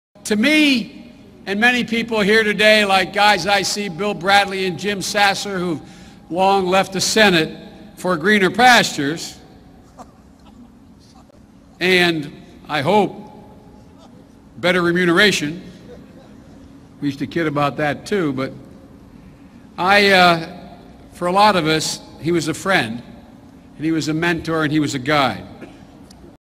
Joe Biden's "friend, mentor and guide" Robert Byrd was an "exalted cyclops" in the KKK.
As a young man in West Virginia, [Robert] Byrd recruited members to a local KKK chapter and was elected to the post of “exalted cyclops” according to his 2005 autobiography. Later, in 1964, when Byrd had become a Democratic U.S. senator, he stalled and opposed major civil rights legislation. - Associated Press